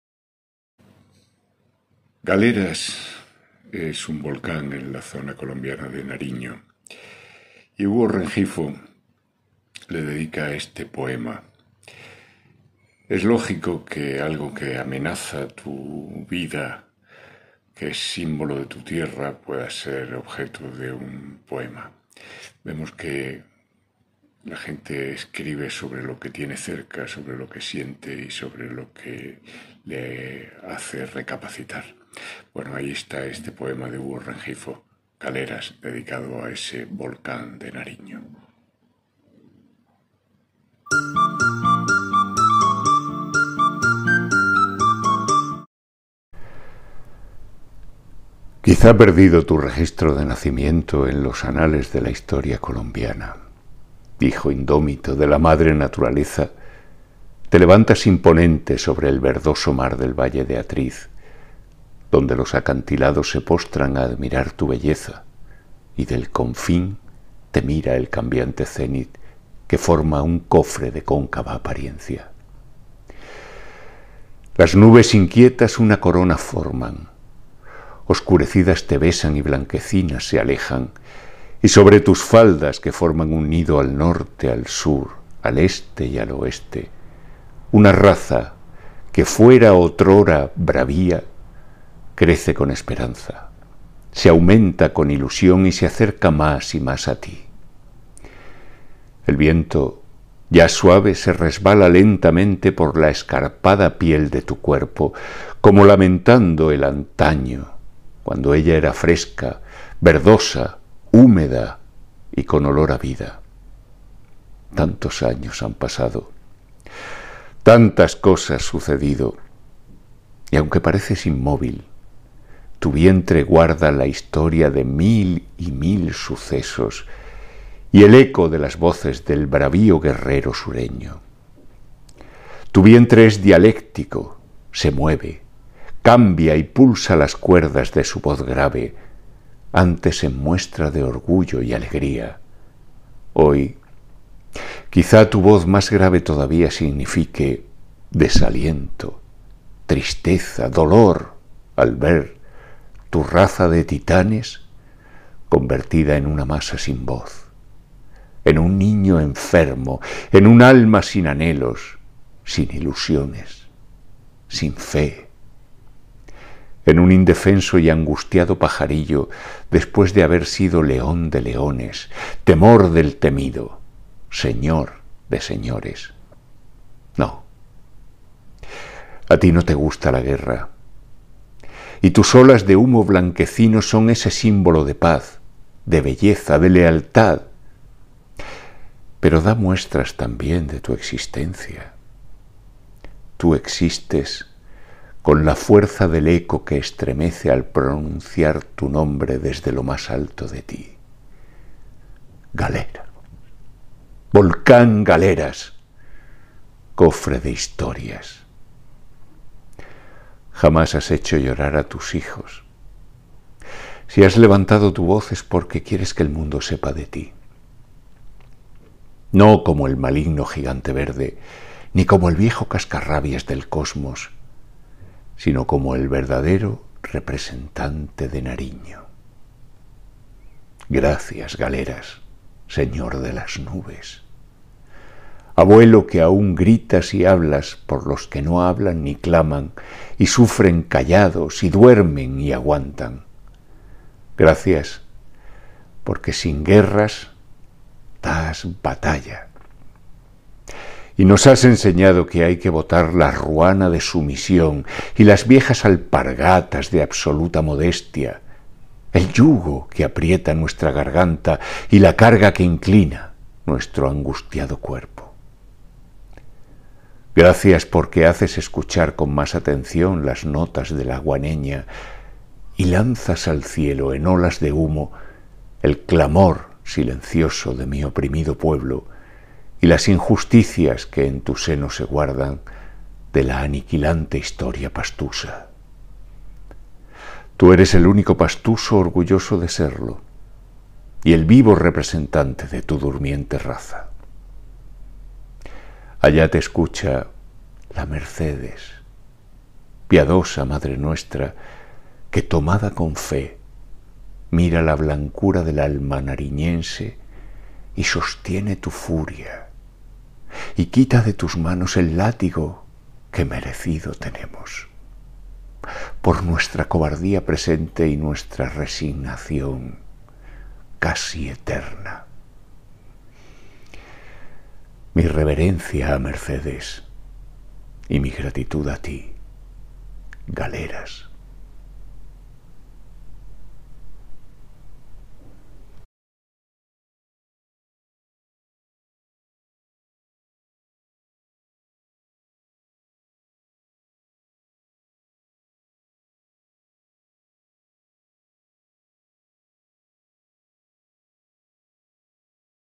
"GALERAS" de Hugo Rengifo Davila.
El volcán Galeras es probablemente el mas activo de Colombia. Situado en el llamado Cinturón de Fuego del Pacifico sirve de recordatorio a los habitantes de Pasto en Nariño de la pequeñez de ser humano contra las fuerzas de la Naturaleza.